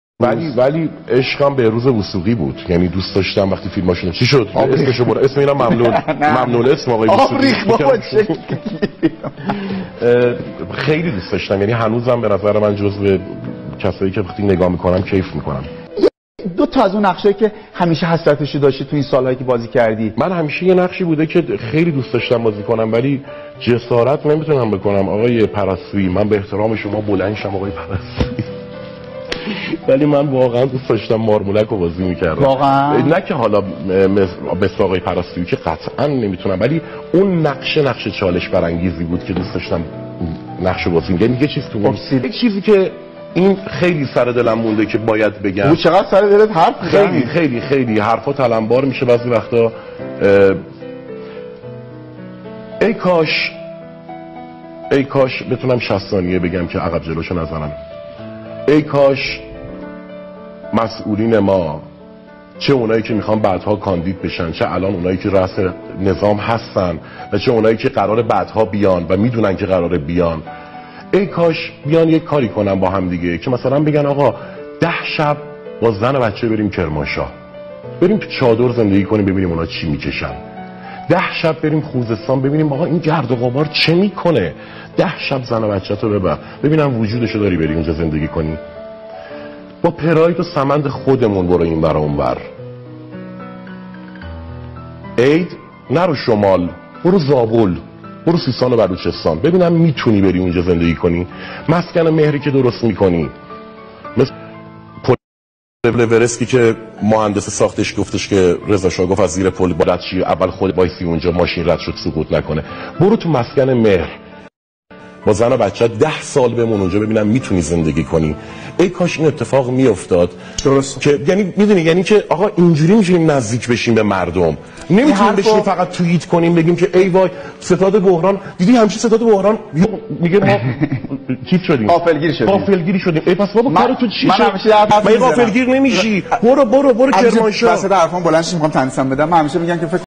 ‏‎امیر جعفری رو آنتن زنده خطاب به مسئولین صدا سیما. اگر وجودش رو دارید پراید سوار شید! اگر وجودش ر
‏‎امیر جعفری رو آنتن زنده خطاب به مسئولین صدا سیما.

اگر وجودش رو دارید پراید سوار شید! اگر وجودش رو دارید برید کرمانشاه توو چادر زندگی کنید!